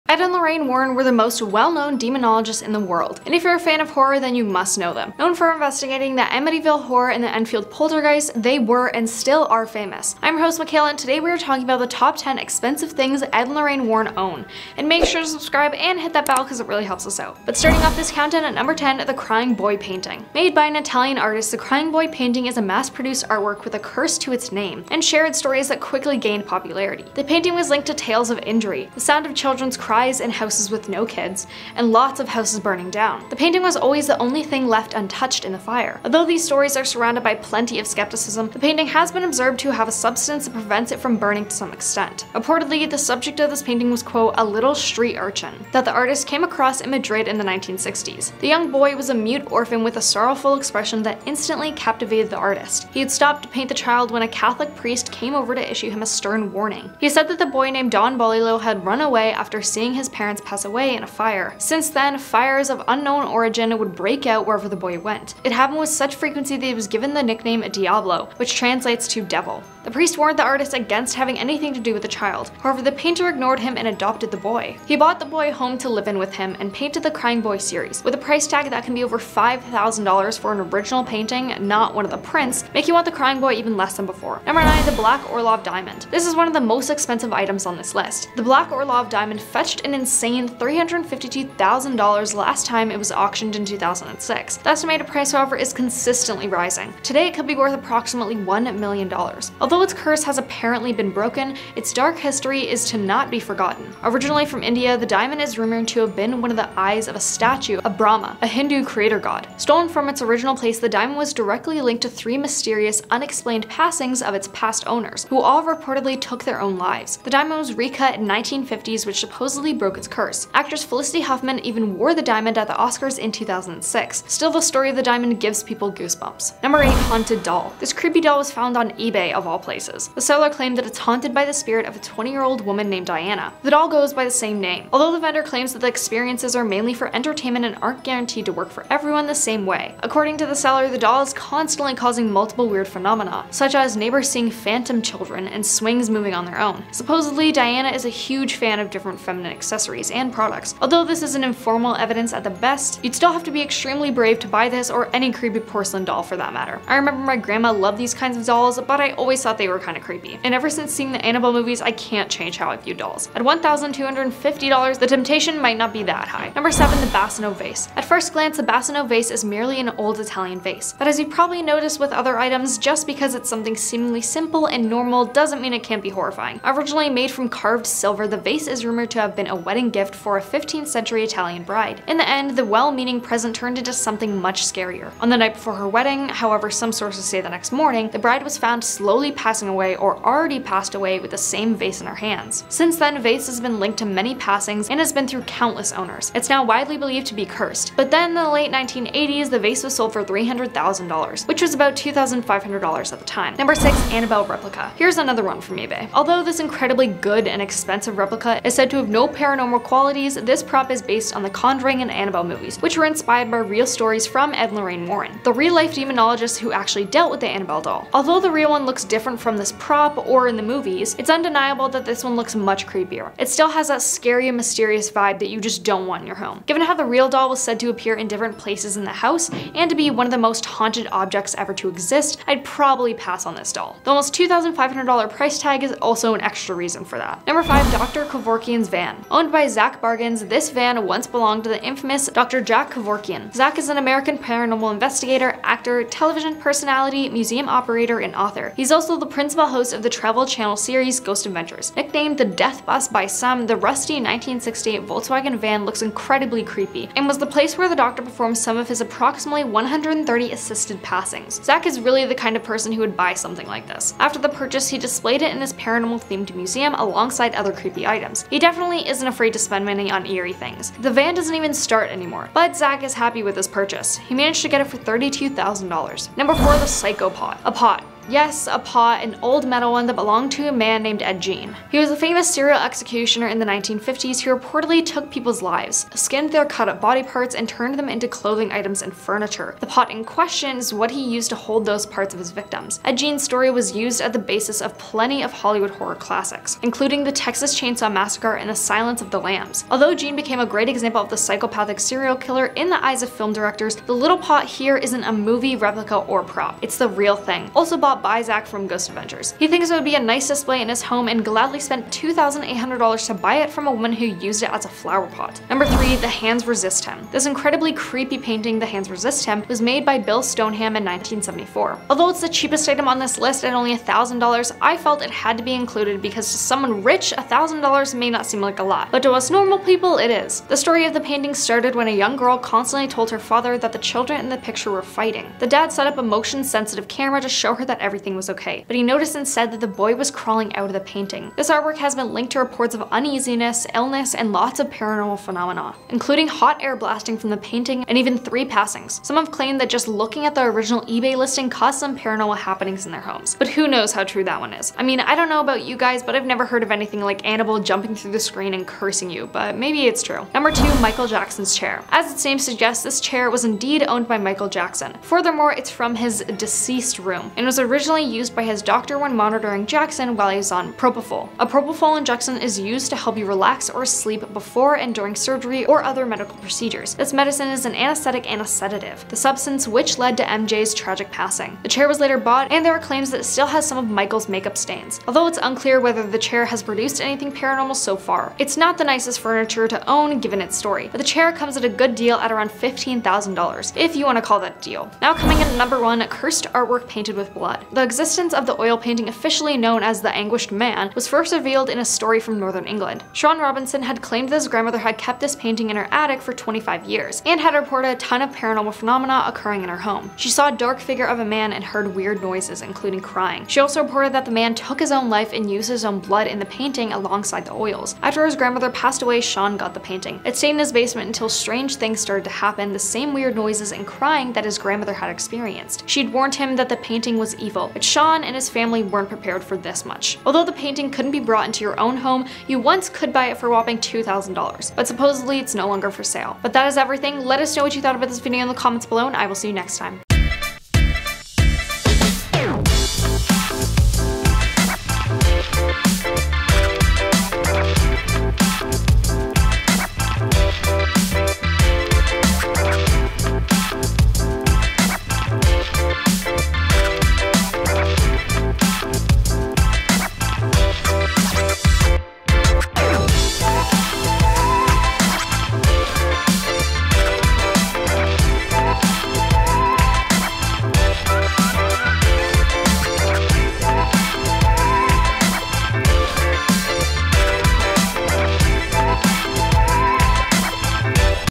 Top 10 Expensive Things Ed & Lorraine Warren Own
Ed Warren and Lorraine Warren were American paranormal investigators and authors associated with prominent cases of alleged haunting such as the true stories behind Annabelle, The Conjuring and more. Being a demonologist however doesn't mean you're broke. The couple own some of the most expensive and haunted items from history. In todays video we will go over the Top 10 Expensive Things Ed & Lorraine Warren Own.